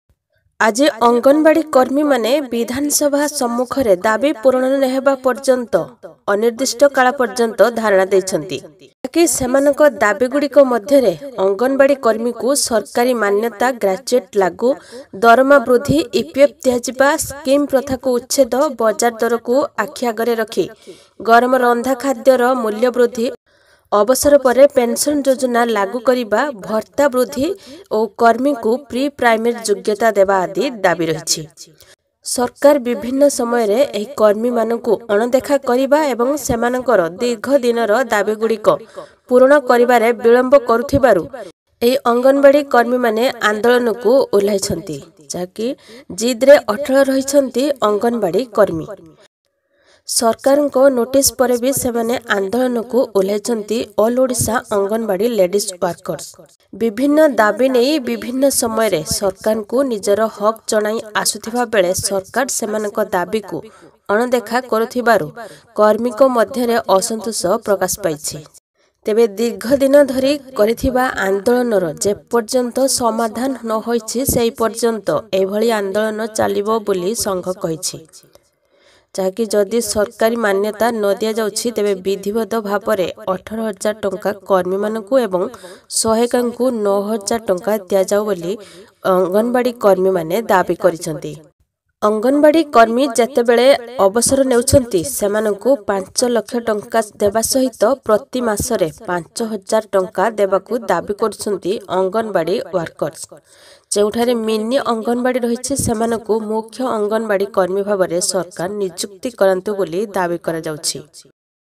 Anganwadi workers protest demanding over various issue | odisha.
Anganwadi workers protest demanding over various issue.